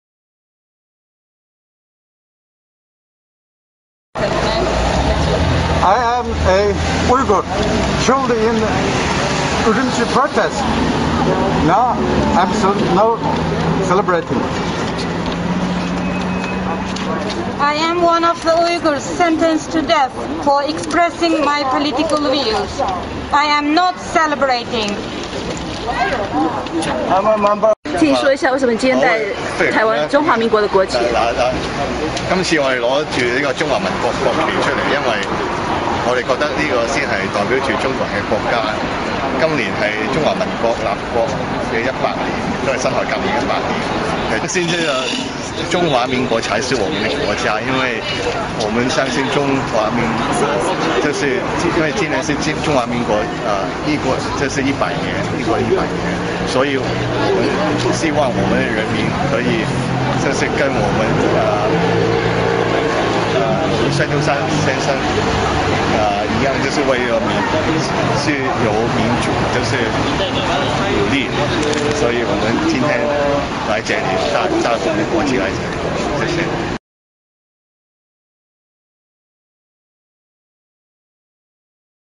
旅英人士举行《十一》不庆祝的抗议活动
许多旅居海外来自中国的人们说，《十一》是不能庆祝的日子，旅英维族歌唱家蕾曼西说，对维族这是国殇日。而一些香港人希望中国人民能和百年前创建中华民国的孙中山一样，为中国的自由民主继续努力。订阅自由亚洲电台YouTube频道

听众谈话

听众谈话